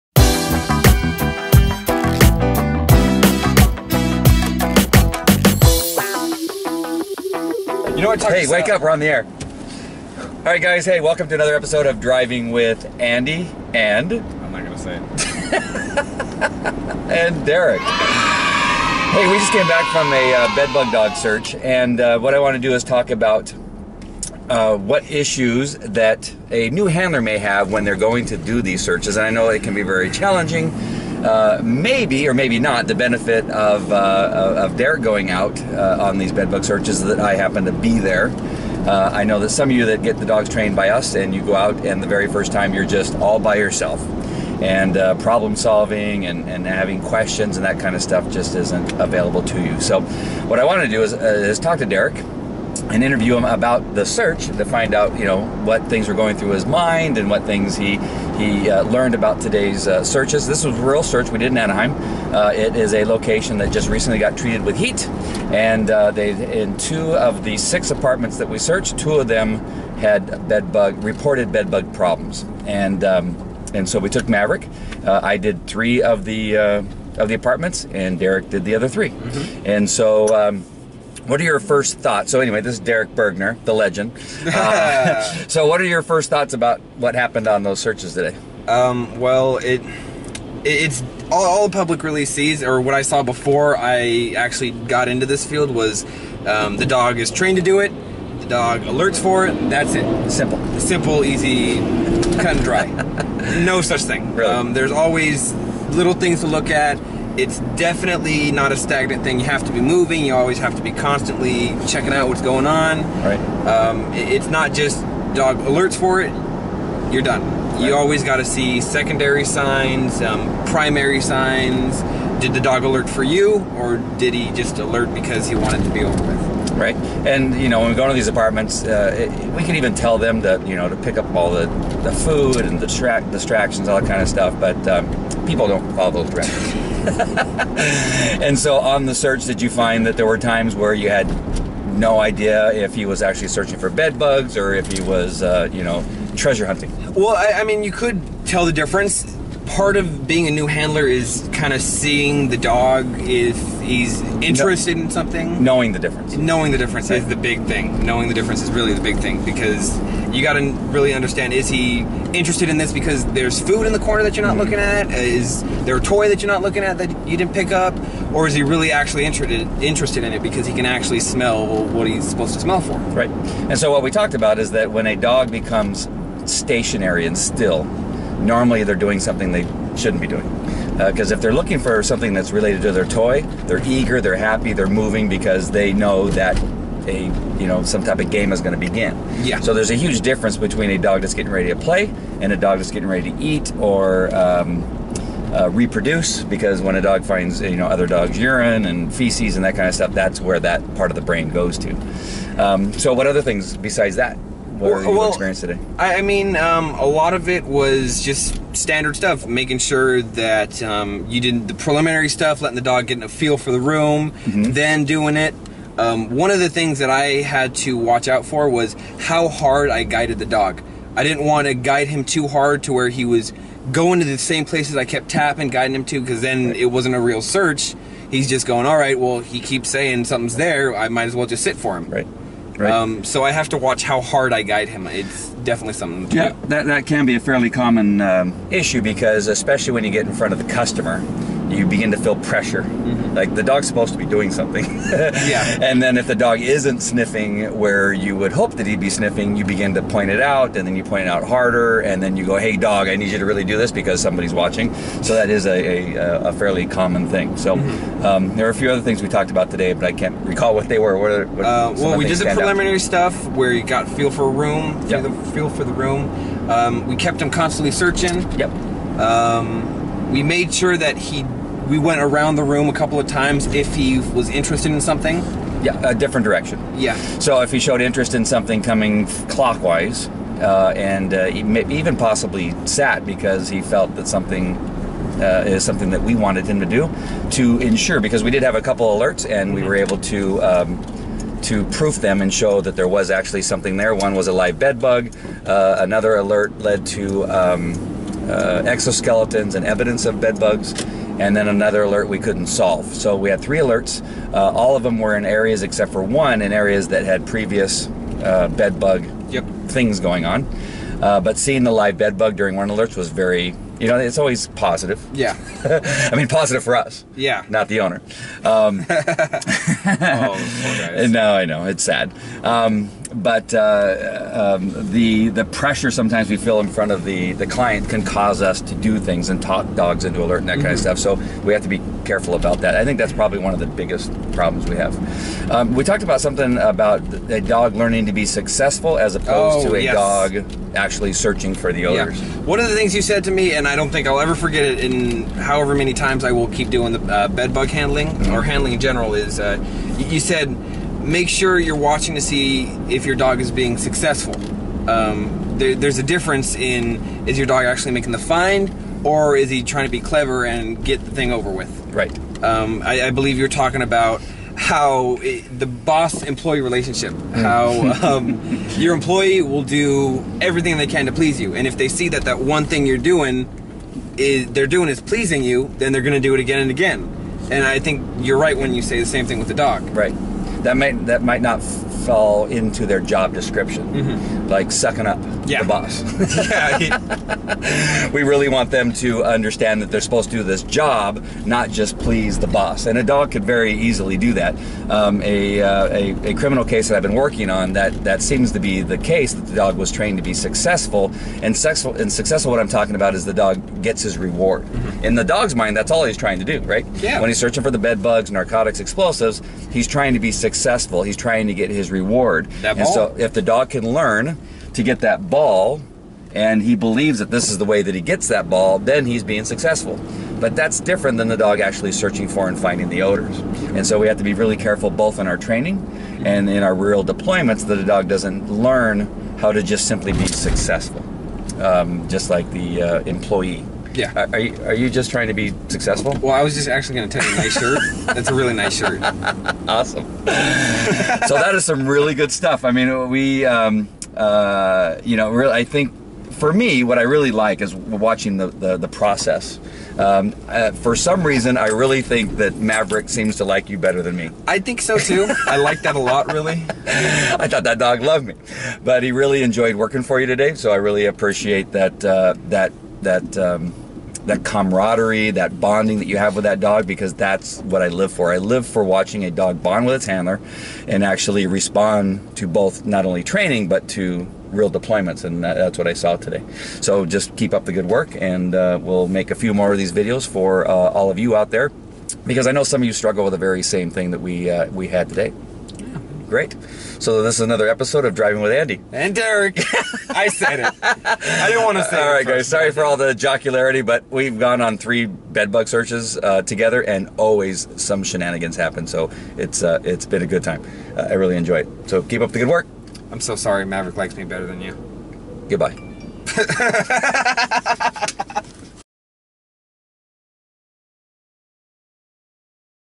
Dog Training for Bed Bug Detection Q&A
Dog Training for Bed Bug Detection. Dog training in Orange County. This episode of Driving with Andy includes Derek Bergner as we leave a Bed Bug Dog search. I took the opportunity to ask Derek about his experience during the search. Handlers of detection dogs will find it interesting... mostly that they are not alone in their thoughts about working with a dog.